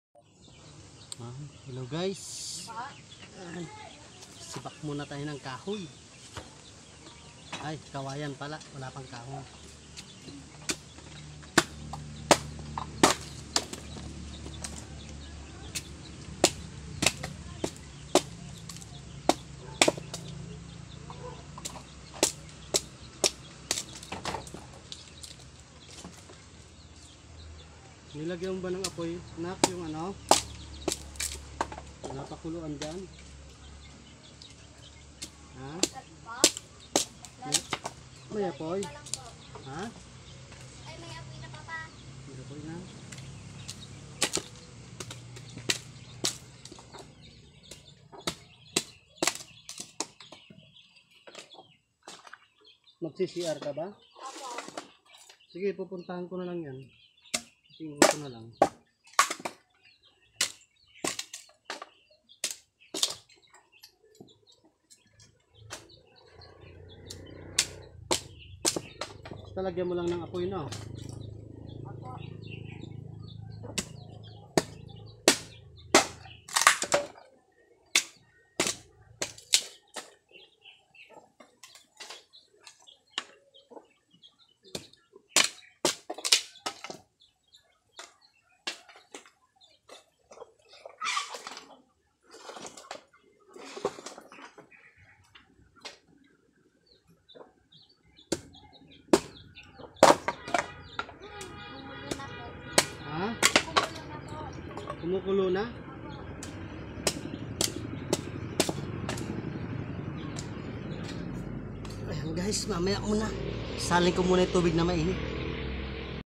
tara mga katanod magsibak muna Ng kahoy para may gamitin sa pagluluto/kawayan pwede rin..
pwedeng pangluto,,gamit sa pagluluto,,upang makapagluti Ng pagkain